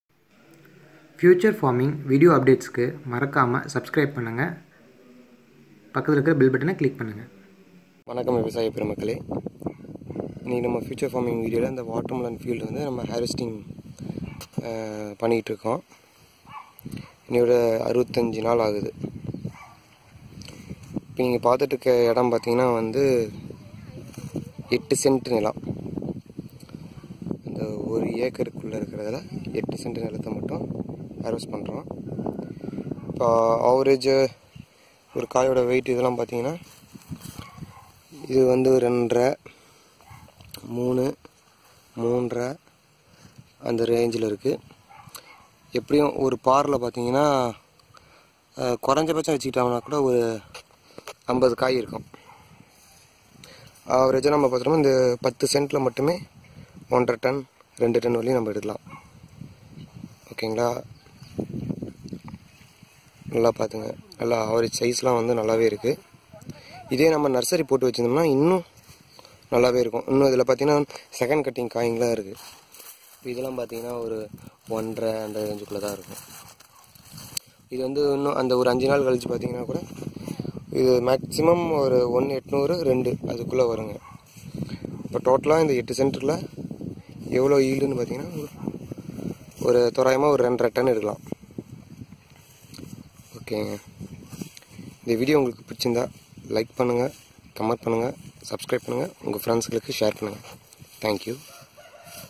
Future Farming || Part 3 (Harvesting) Watermelon cultivation using Drip and Mulching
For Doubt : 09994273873